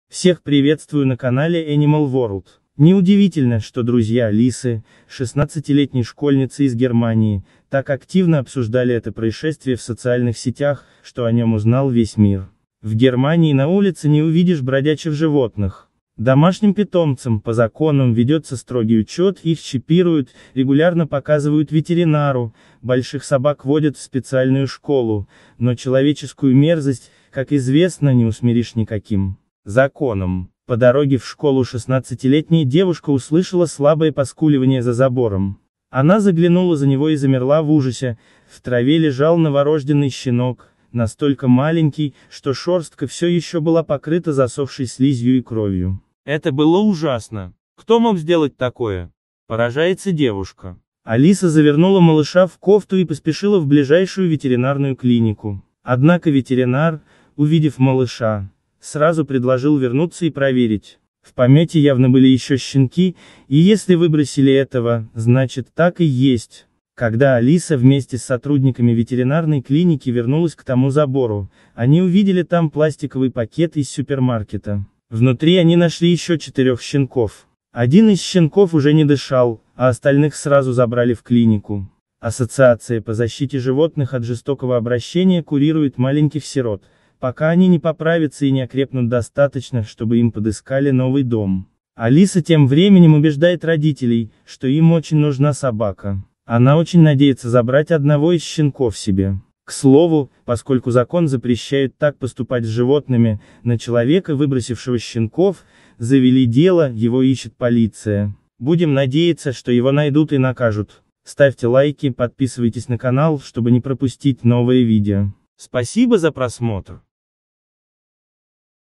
Девочка спасла щенков, выброшенных в пакете
По дороге в школу 16-летняя девушка услышала слабое поскуливание за забором. Она заглянула за него и замерла в ужасе: в траве лежал новорожденный щенок, настолько маленький, что шерстка все еще была покрыта засохшей слизью и кровью.